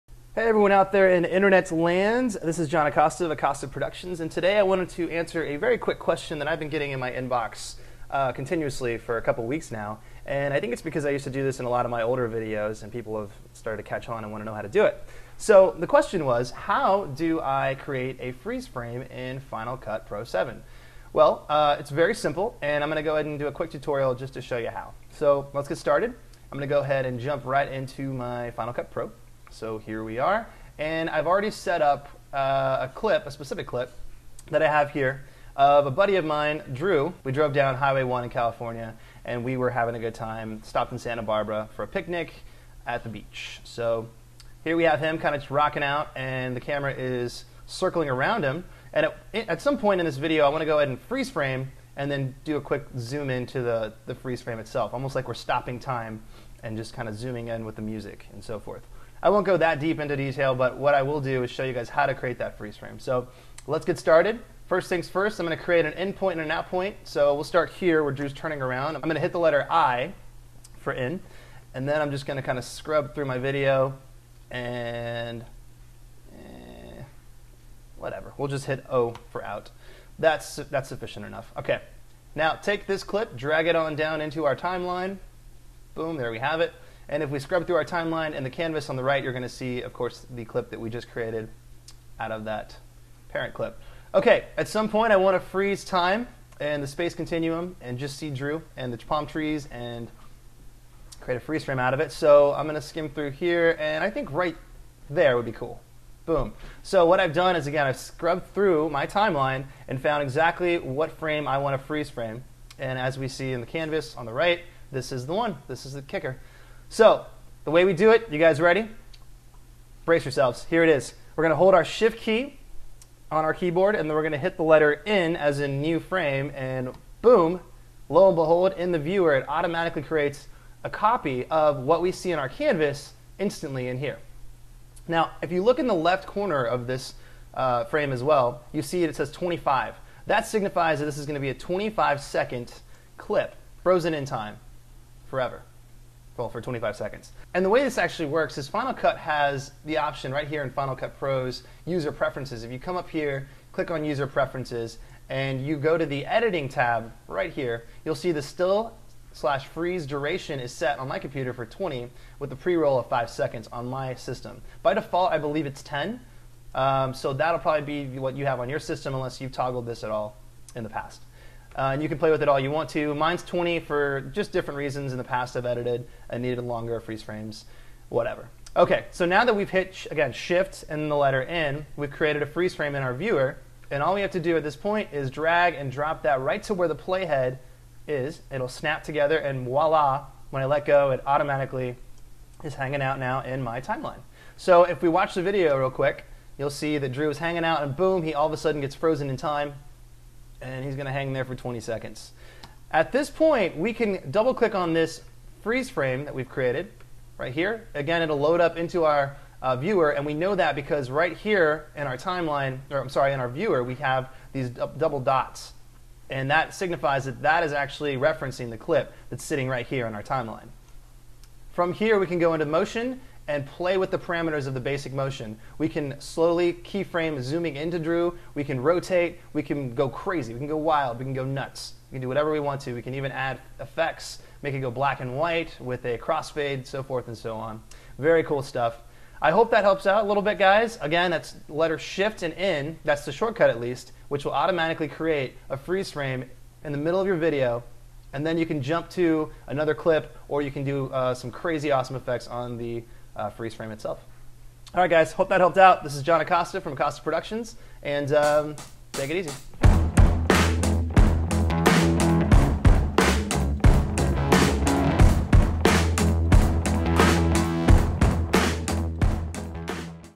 How to make a FINAL CUT FREEZE FRAME - Final Cut Pro 7 Tutorial
Trying to create Freeze Frames in Final Cut Pro? This tutorial shows you how...

Make sure to LIKE if you learned something.

New to DSLR Video?The best DSLR for Video is the Canon 70D. Here's Why